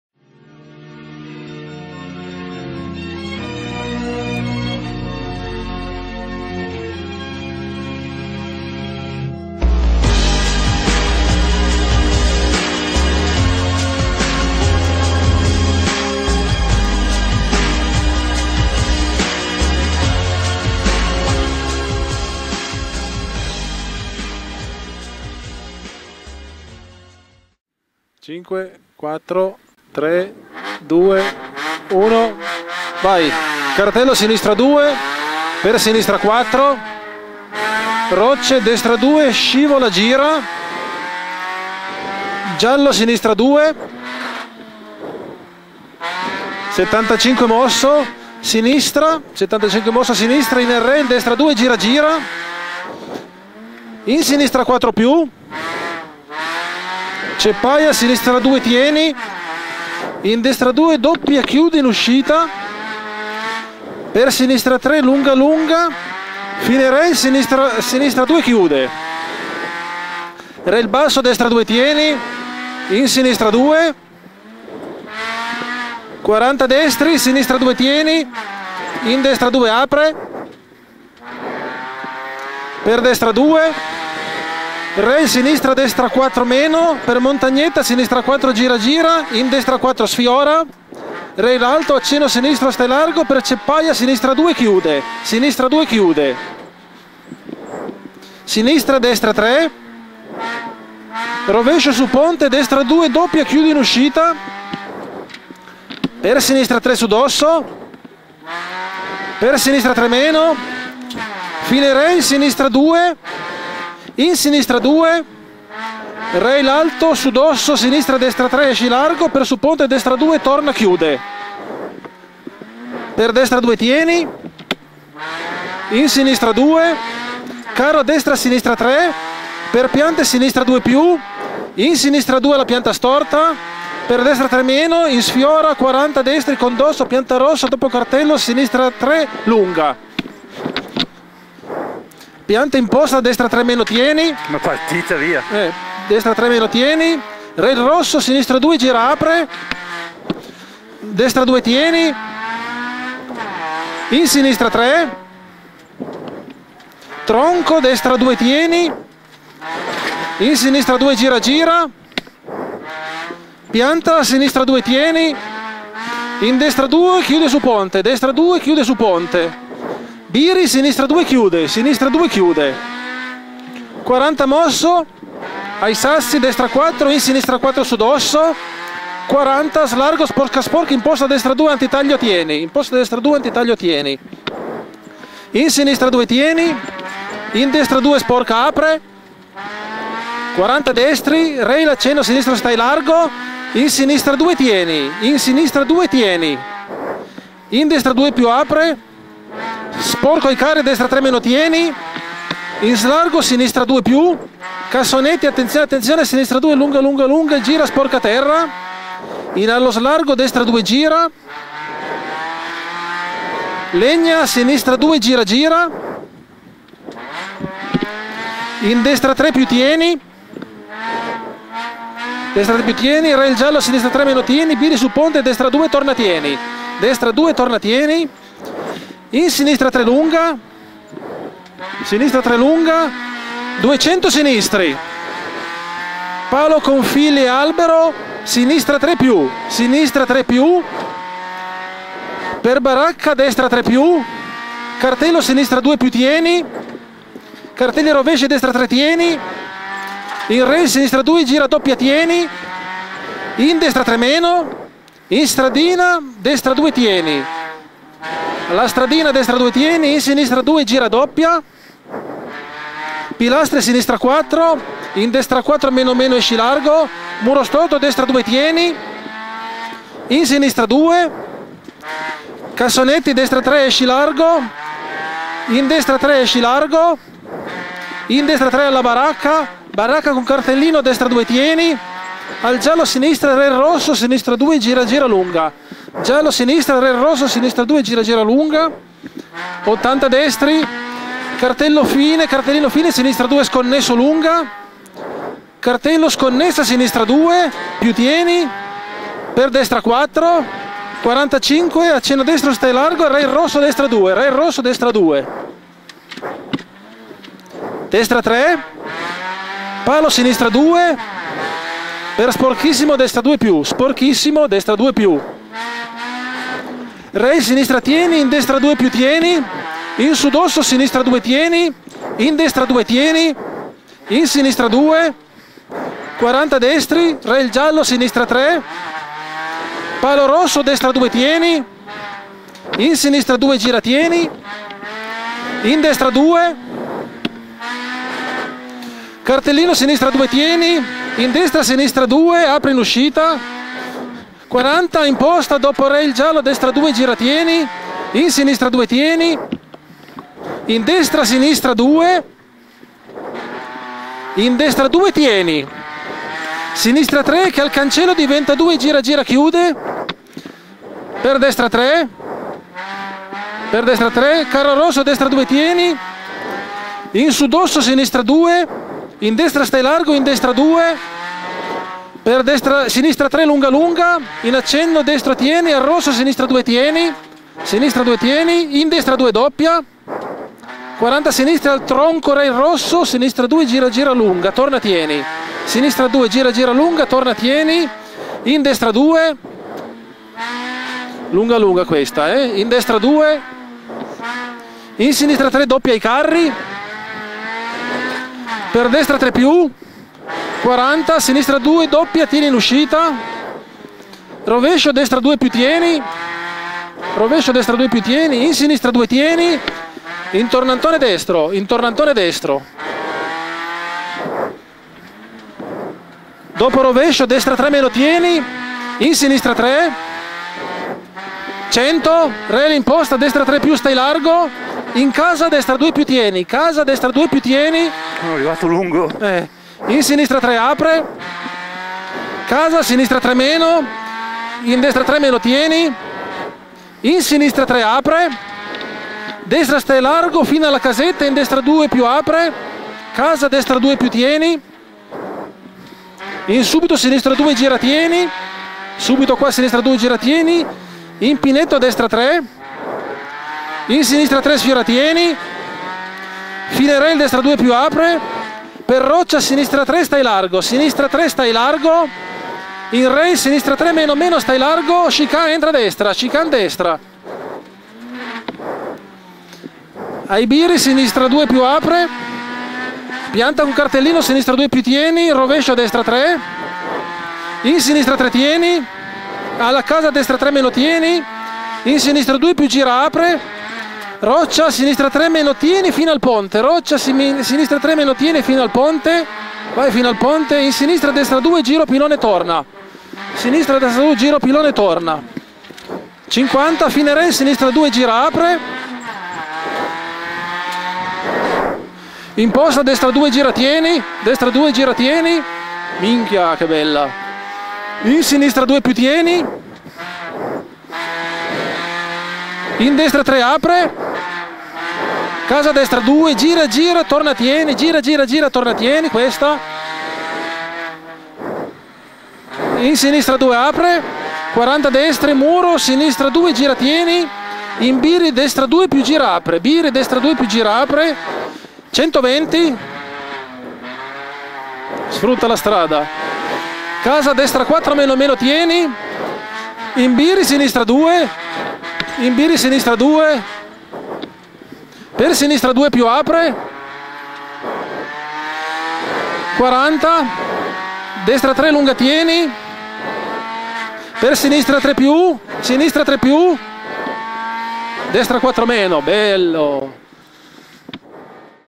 Cameracar Rally della Lanterna 2012 Mandelli-Gremo Swift RS3 3°di classe
Cameracar Rally della Lanterna 2012
Eugenio Mandelli-Alessio Gremo
Suzuki Swift RS3
Cameracar by LucaVideo